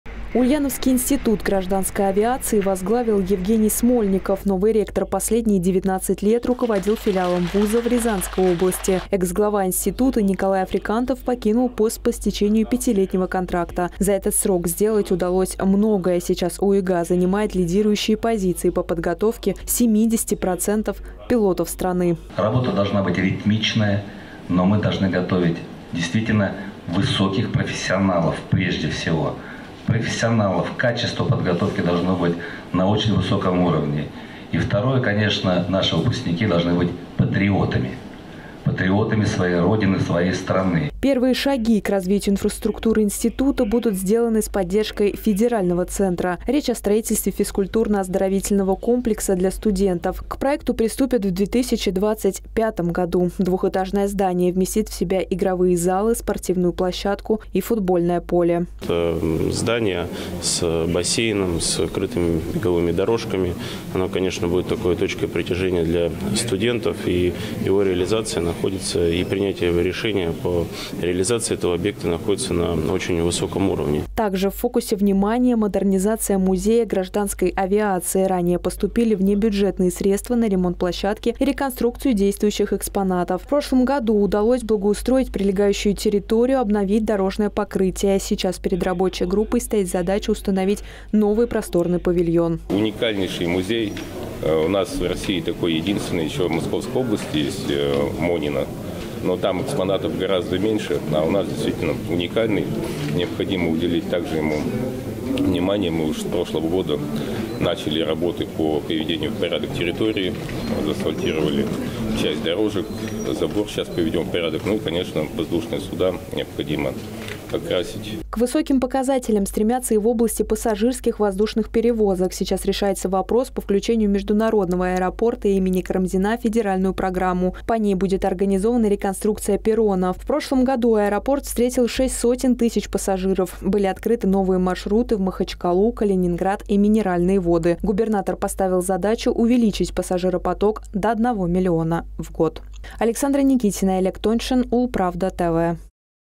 Новое руководство и ремонт. В ульяновском институте гражданской авиации грядут изменения
В институте гражданской авиации — новый руководитель. Им стал Евгений Смольников. Ректора представил руководитель Росавиации Дмитрий Ядров. Экс-ректор Николай Африкантов покинул пост по семейным обстоятельствам. Сейчас перед ВУЗом стоят новые амбициозные планы – строительство современного ФОКа для студентов.